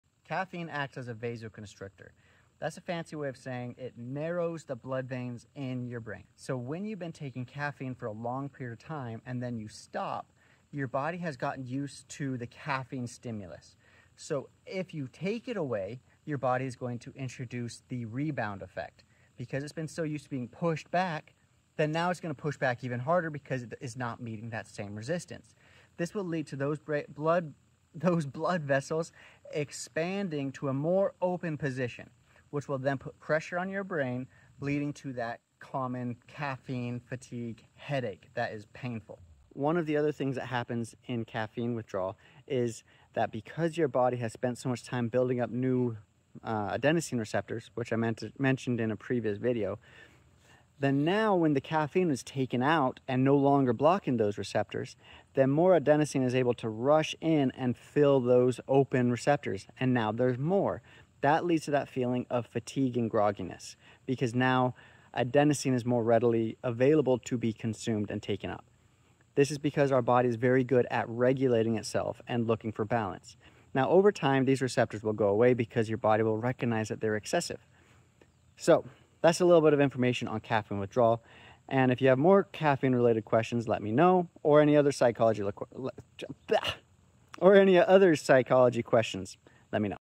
Caffeine Withdrawal Headache: EXPLAINED! (Science)
Why does skipping your morning coffee lead to that brutal headache? 😫 Dive into the fascinating science of caffeine withdrawal headaches! Learn how caffeine impacts blood vessels in your brain, leading to vasoconstriction and then painful vasodilation when you stop. Understand the physiological mechanism behind this common symptom and get tips on how to taper off caffeine to avoid the severe headache and fatigue.